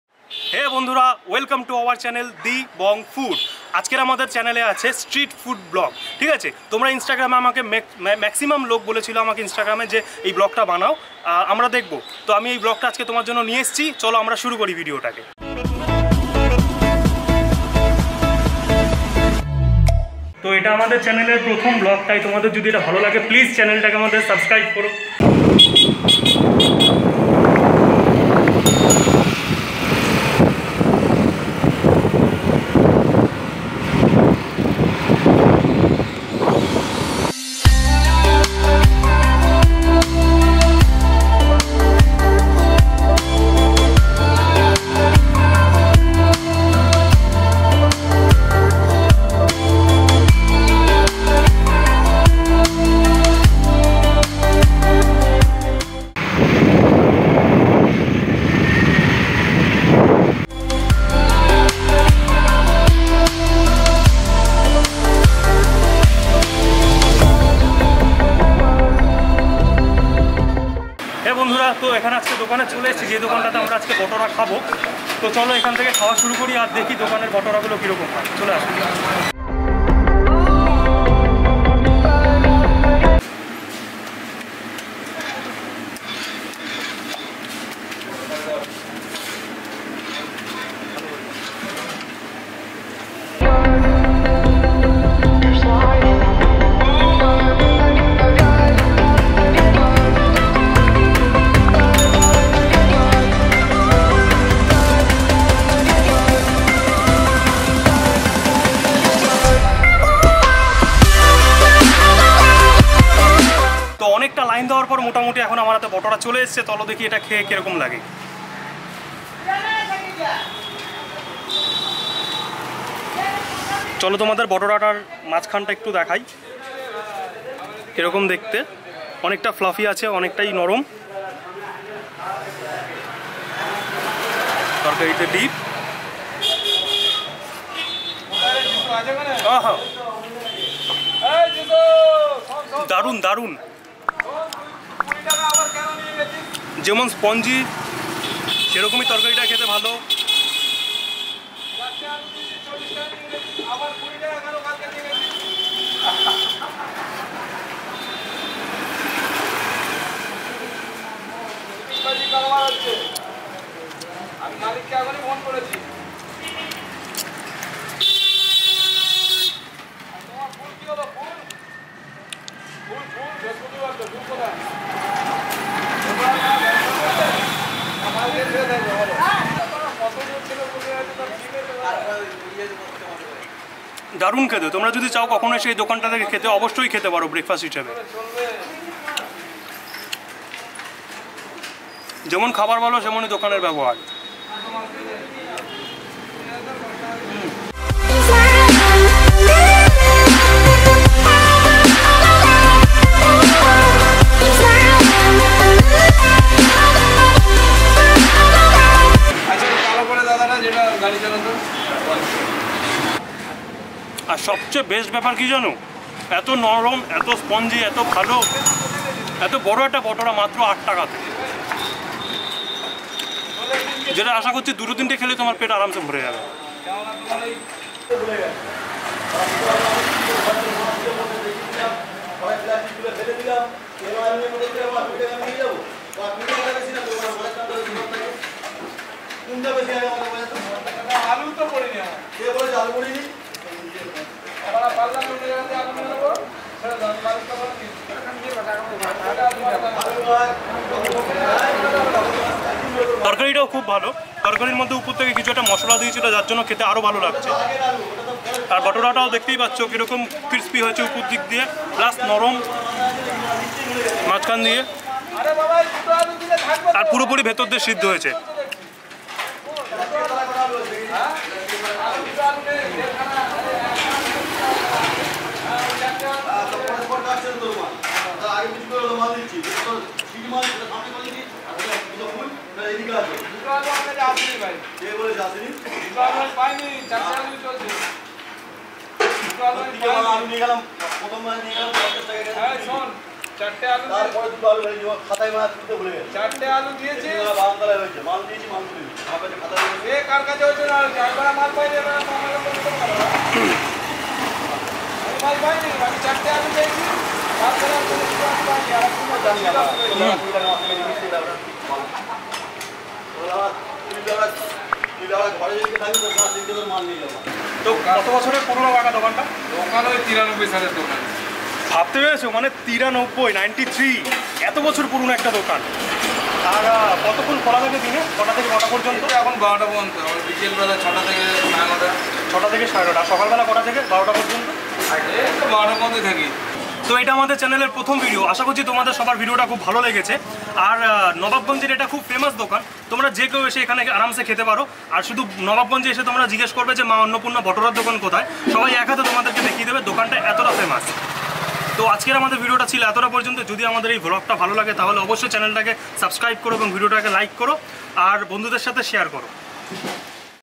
Bhature Eating Vlog | Breakfast in Just 8 Rupee | Indian Street Food Vlog Show
The Shop Location - Maa Annopurna Botora Center.
                                     Ichapur Nawabganj , Beside of Nawabganj Library.
 Opening Time - Morning 7.30 to 12 pm
                             Evening  6.30 to 9.30 pm.

We all love Bhature Eating Vlog so that's why i make this video. Maximum people was told me that i should make the Indian Street Food Vlog Show .